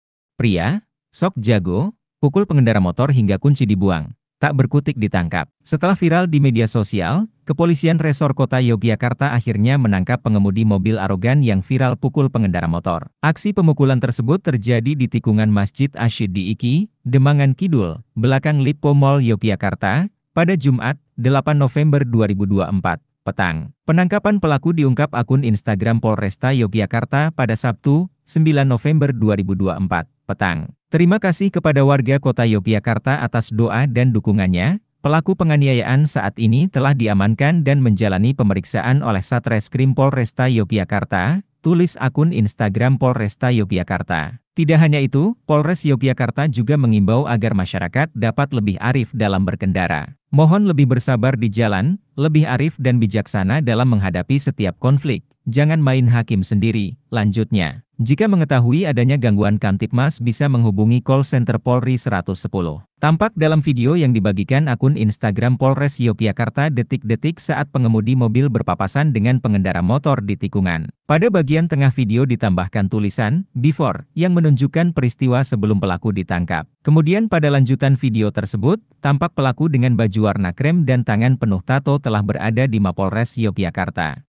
Pria 'Sok Jago' Pukul Pengendara Motor Hingga Kunci Dibuang, Tak Berkutik Ditangkap
Aksi pemukulan tersebut terjadi di Tikungan Masjid Ashshiddiiqi, Demangan Kidul, belakang Lippo Mall Yogyakarta, pada Jumat (8/11/2024) petang.

Editor: Muhammad Maulana Ahmad Al H